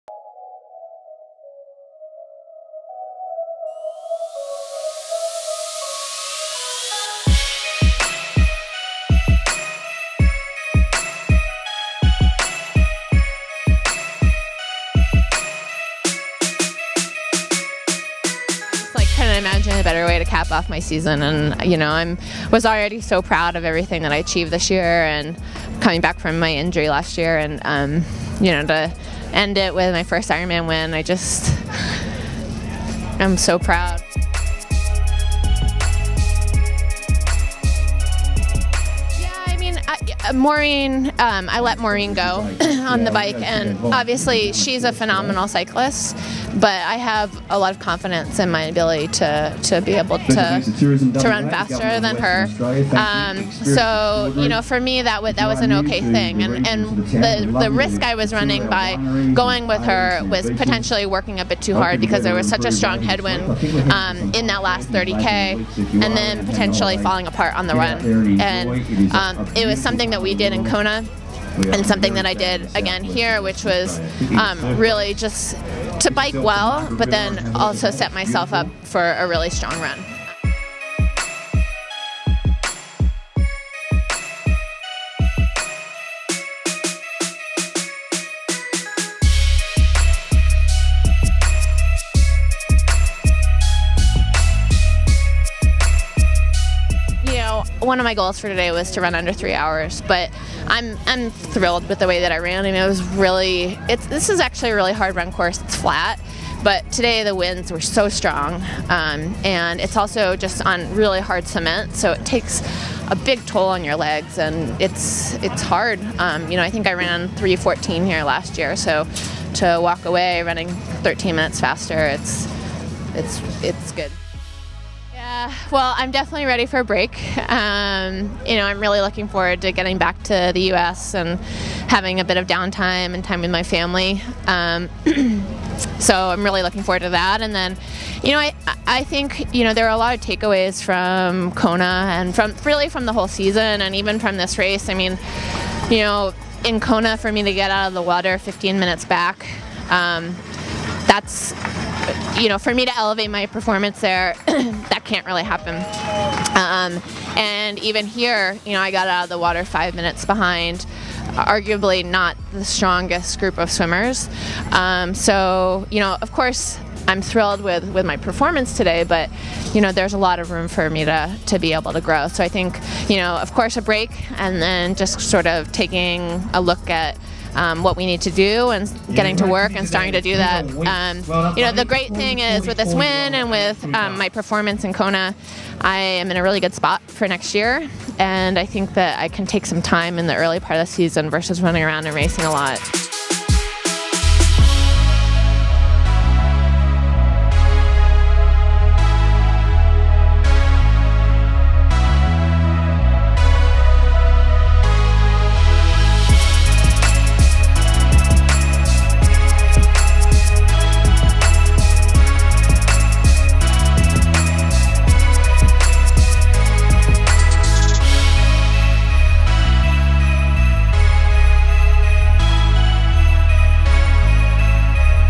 Sarah Piampiano Wins Ironman Western Australia
Witsup was there to capture the action at Ironman Western Australia, which saw American Sarah Piampiano claim her first Ironman win after a huge year of racing since her injury in 2014.

Includes race day footage of the top five women and an interview with Piampiano.

** If you want to share this content, please feel free, but be cool and credit/tag us appropriately. **